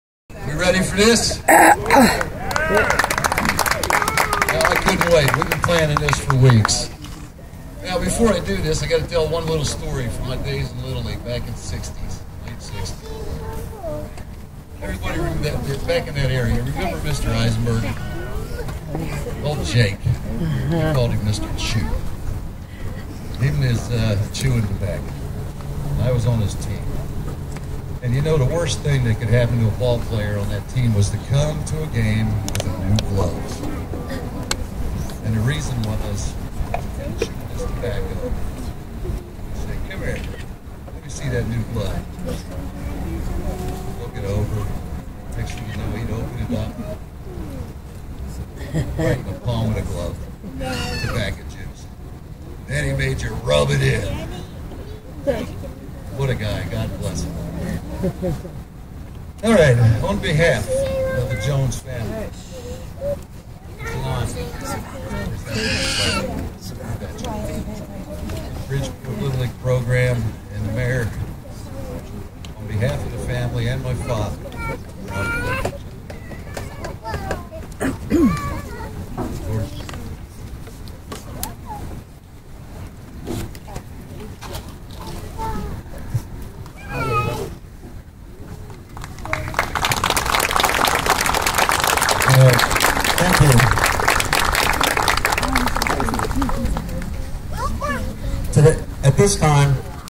Bridgeport Little League Alumni Ceremony 3
Presentation of the flag in honor of Bob Jones by The Jones Family (Part 2)